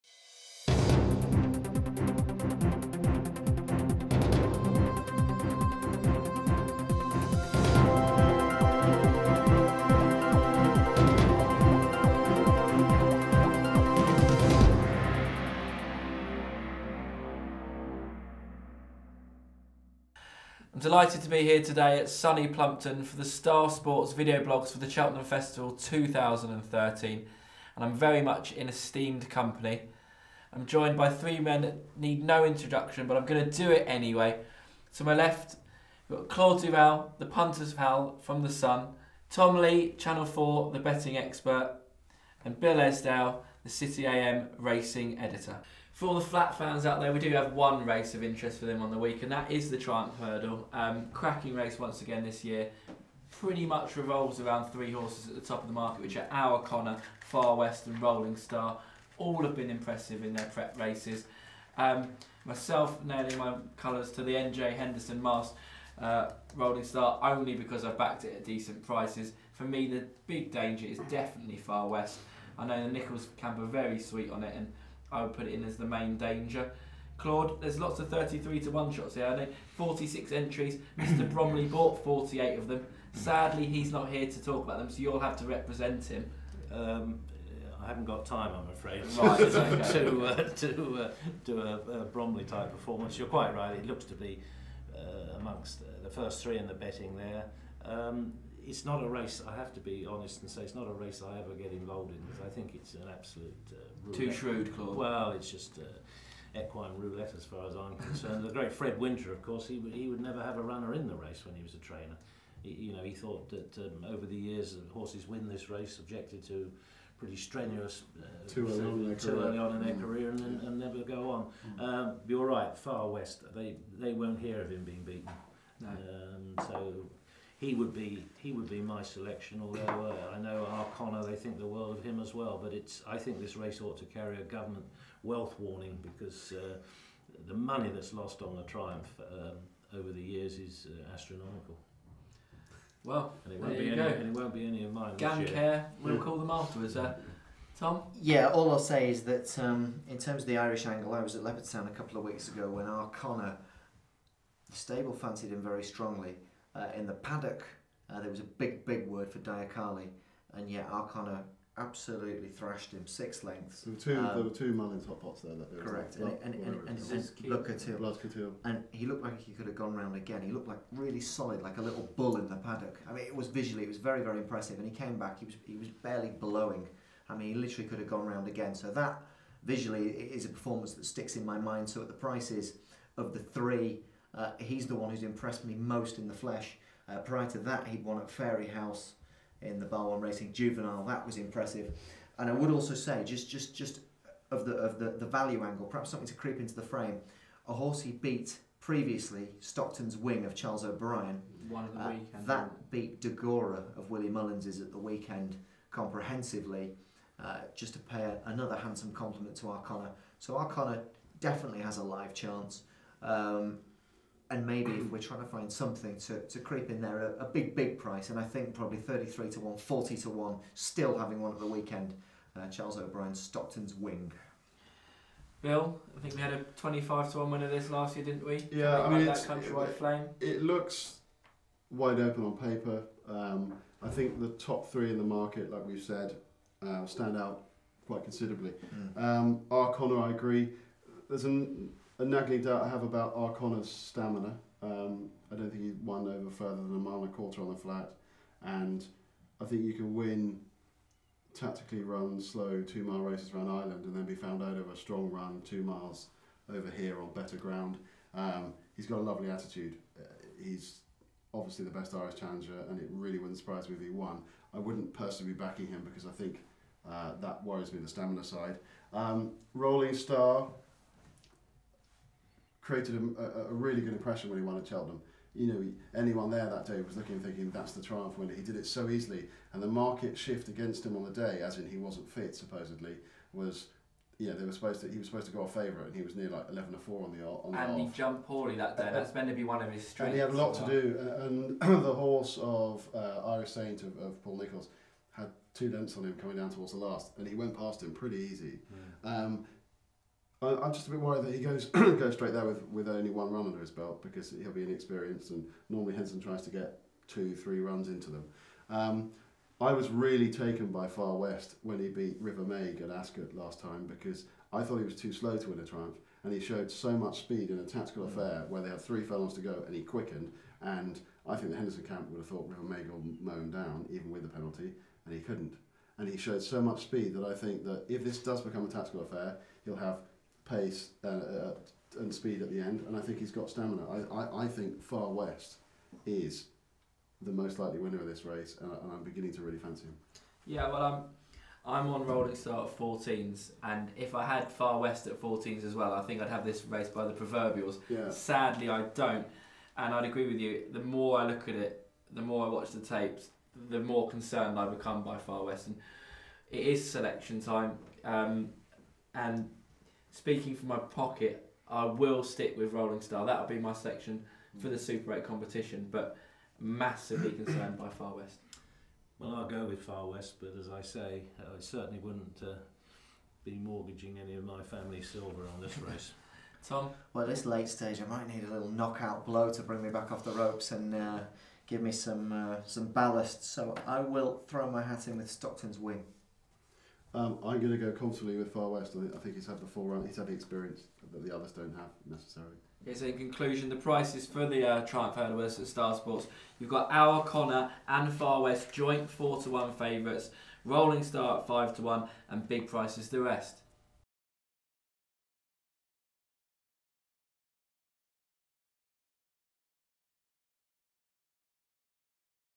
The Triumph Hurdle - Cheltenham Festival Preview 2013
Our Head of PR, Luke Tarr is joined by Claude Duval, The Punters Pall (The Sun), Tom Lee Channel 4 and Bill Esdaile City Am Racing Editor. The panel preview the Triumph Hurdle in this years Cheltenham Festival 2013.
Produced by Ivan Seka + Nicholas Jessup
Directed by Sam Franklyn
Sound by Rowan Bishop
Filmed and edited by Concrete Rose Productions.